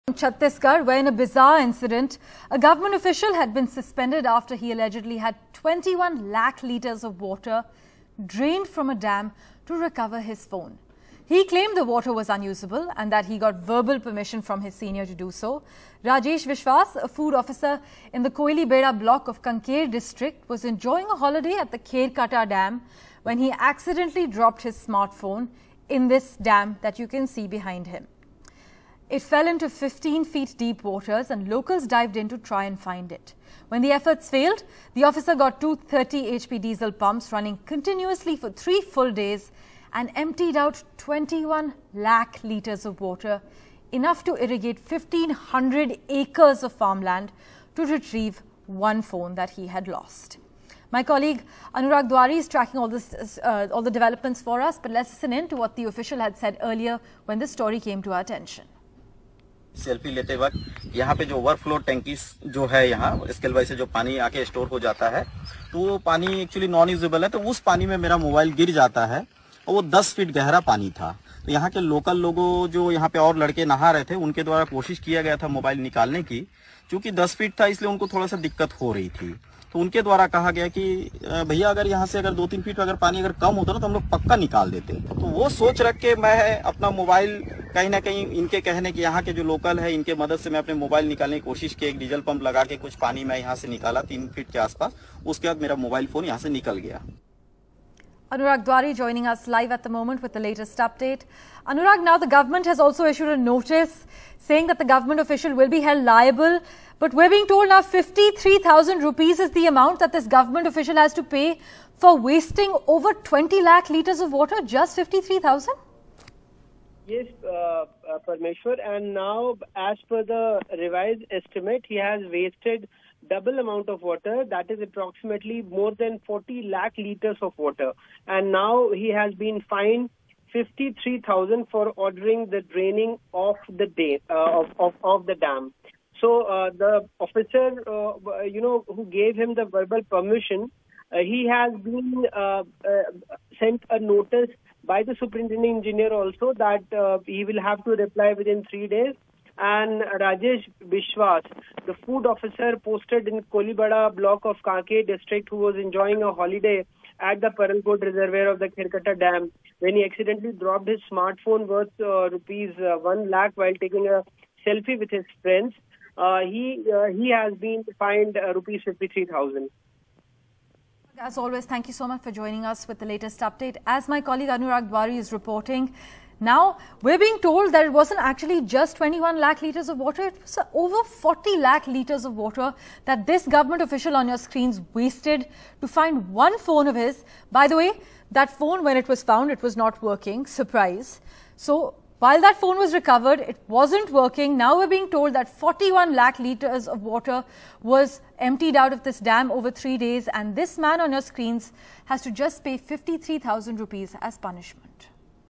Officer Fined Rs 53,000 For Emptying Dam In Search Of Colleague's Phone
Days after a Chhattisgarh food inspector was suspended for having 42 lakh litres of water drained from a reservoir to recover his expensive phone, the government pulled up his senior, who he said gave him verbal permission to empty water up to five feet. The senior officer has been fined ₹ 53,000 for ordering the draining of the dam.